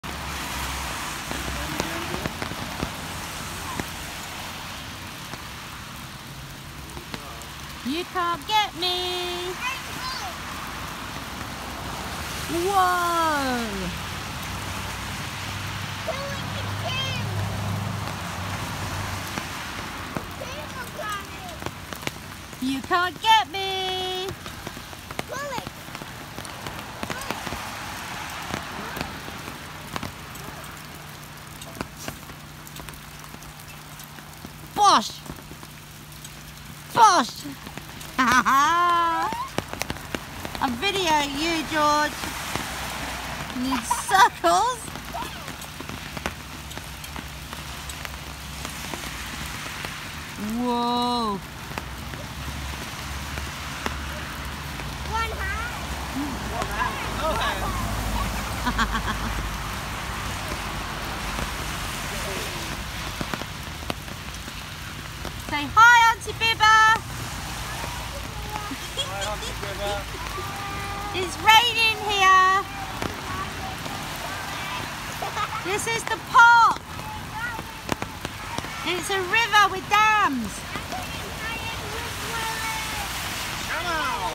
Spiderman on the swing
Pouring with rain but Spiderman still has to spin his web on the swing!